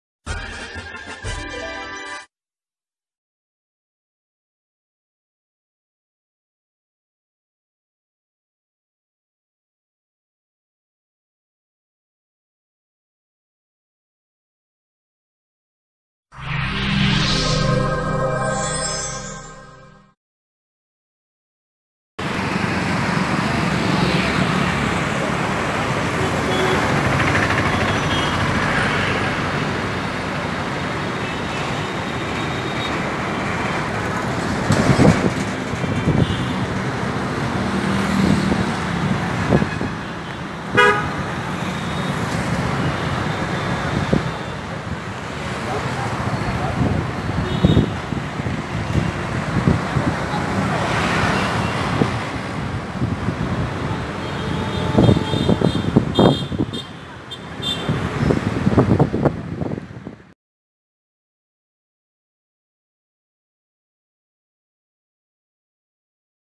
Secunderabad centre shopping mall ,Neredmet Secunderabad Telangana
Secunderabad centre a shopping mall located at Neredmet Secunderabad Telangana outer view